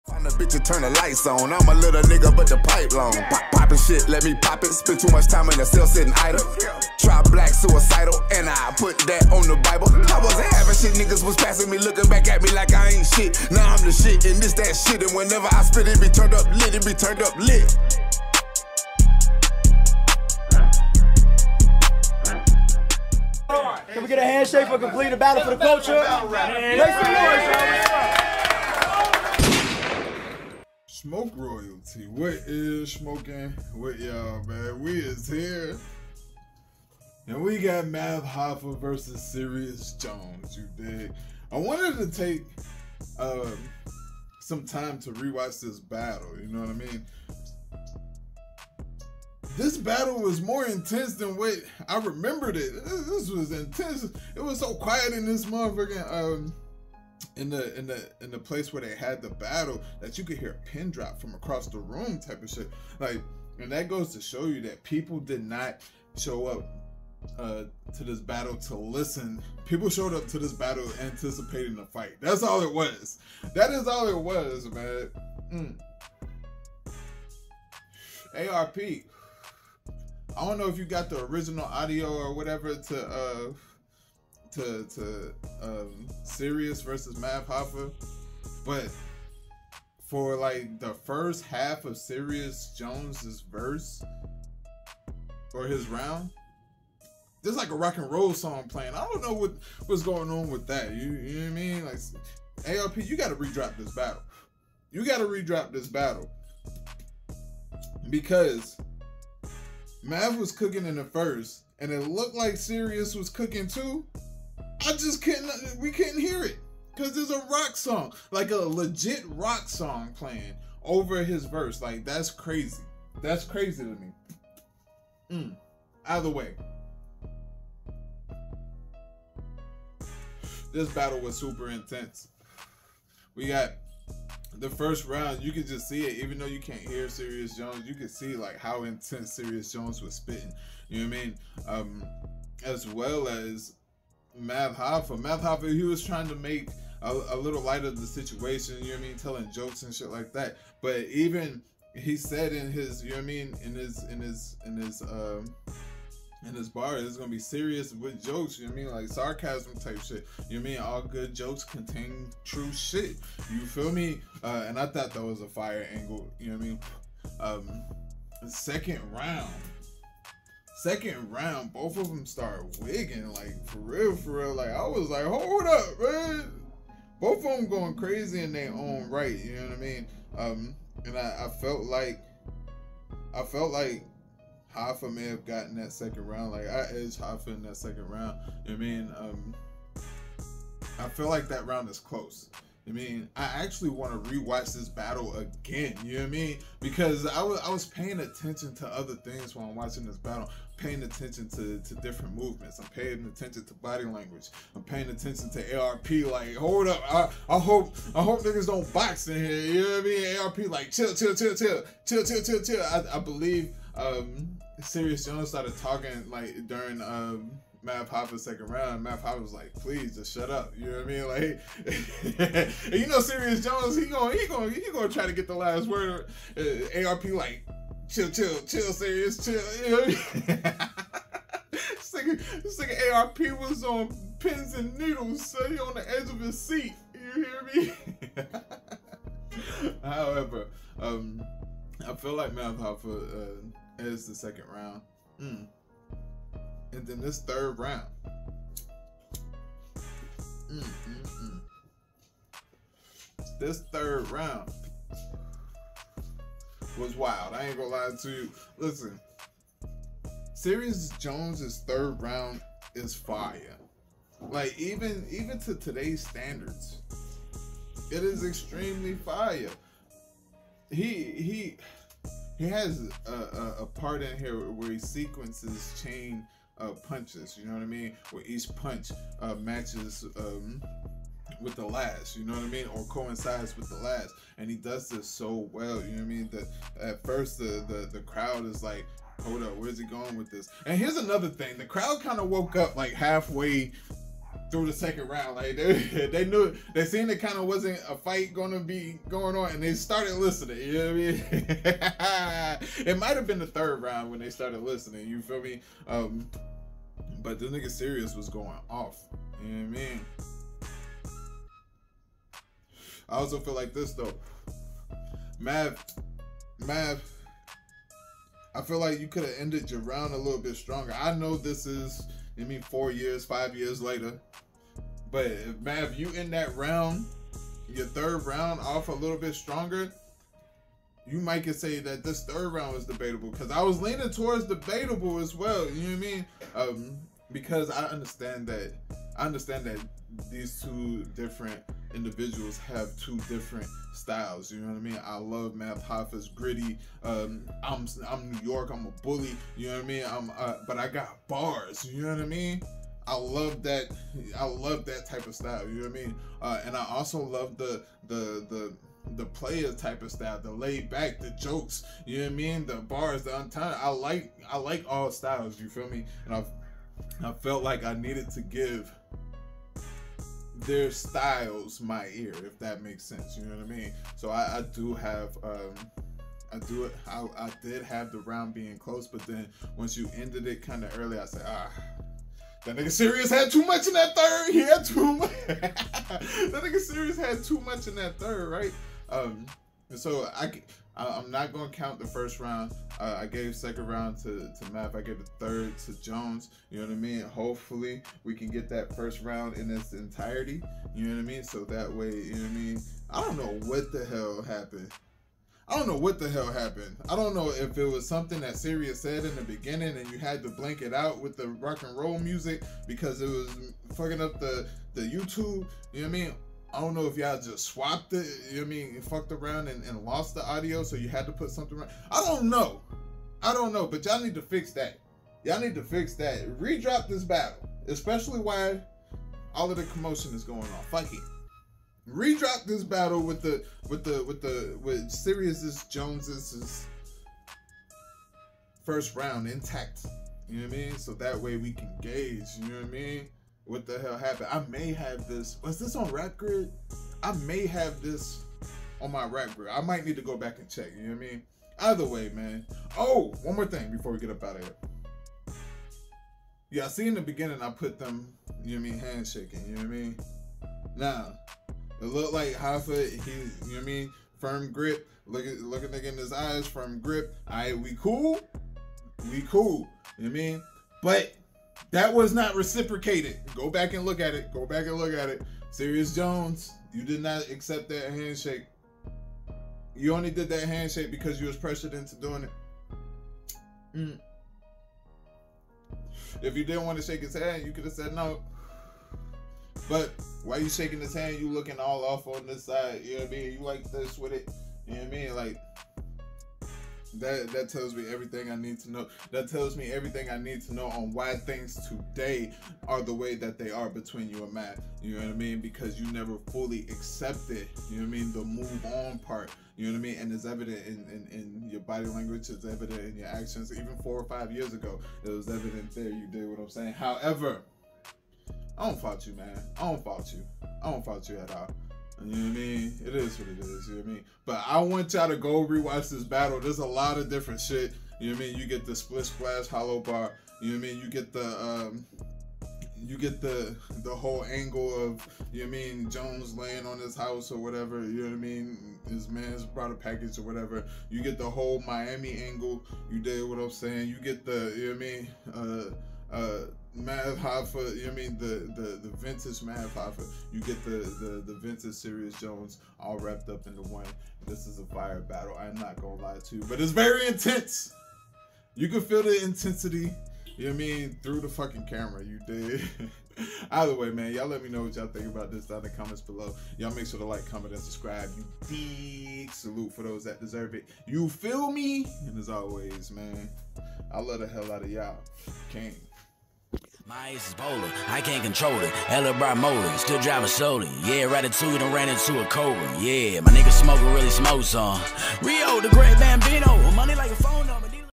Math Hoffa VS Serius Jones Recap: RBE's Closure Battle Raps Grudge Match!
Smok3KingSTV Math Hoffa vs. Serius Jones: RBE's Closure Battle Rap Grudge Match!

Join me as I delve into the intense grudge match between Math Hoffa and Serius Jones at RBE's Closure event.

This battle was electric, with both artists bringing raw energy and undeniable tension to the stage.

Dive into the breakdown of this high-stakes clash, exploring the heated exchanges and memorable moments that made this battle a standout in Battle Rap history

Witness the tension unfold in this intense Math Hoffa vs. Serius Jones battle!